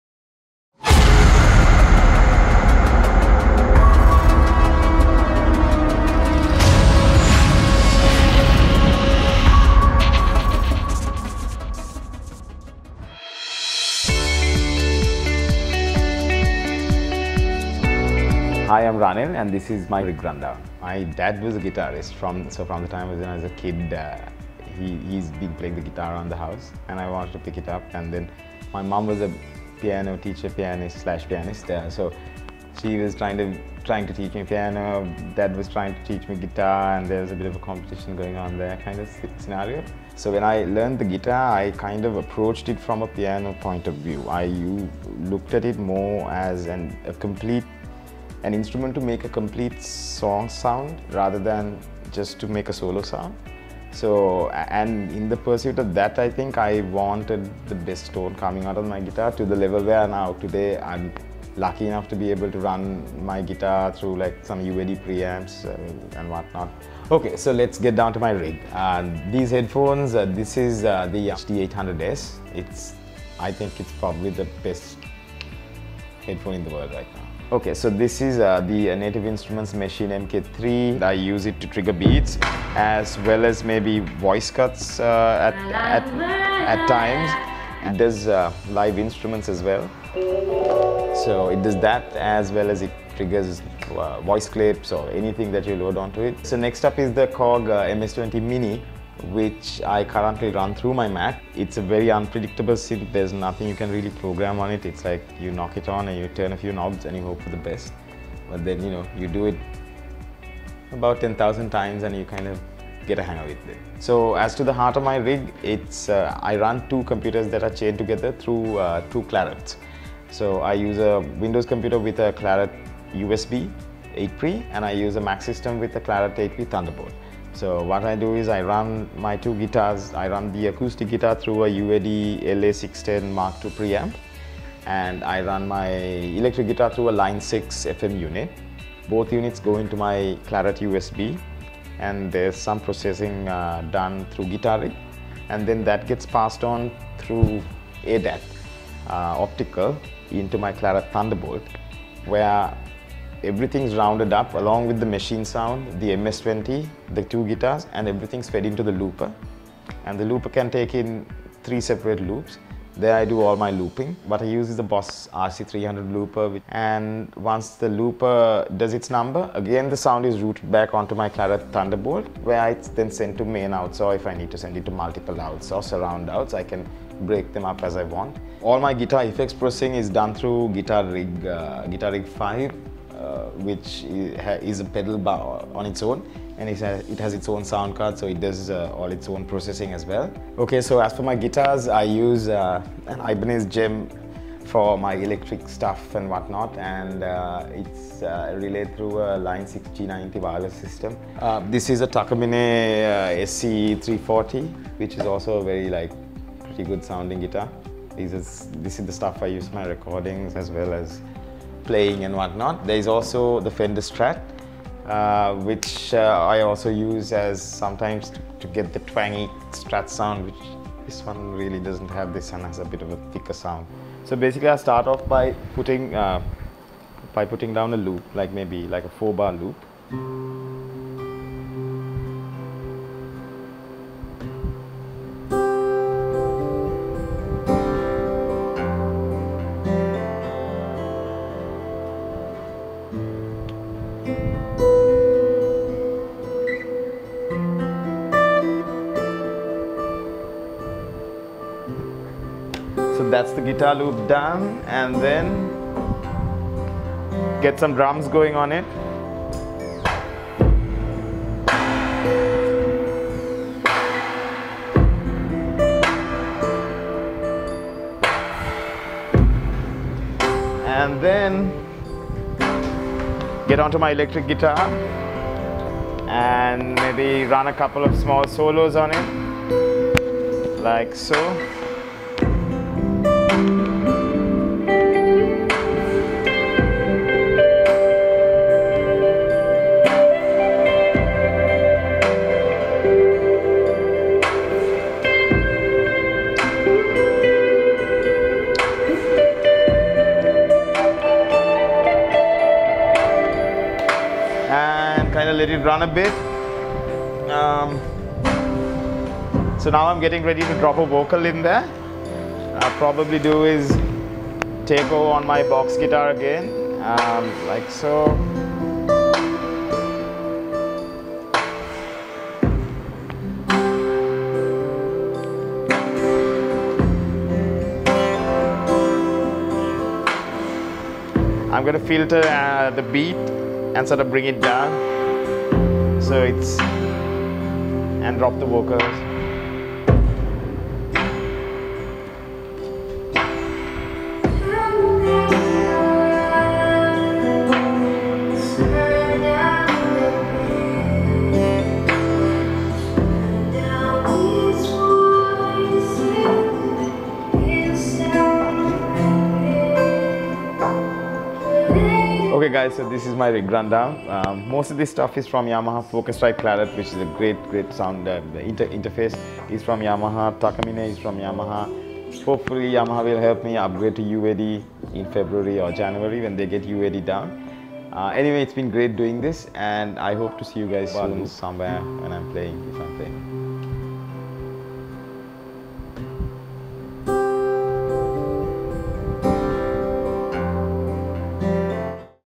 Rig Run Down - Ranil Goonawardene
Ranil has been composing and producing music for almost 2 decades now and in that time he had carved out a sound that is truly unique to himself.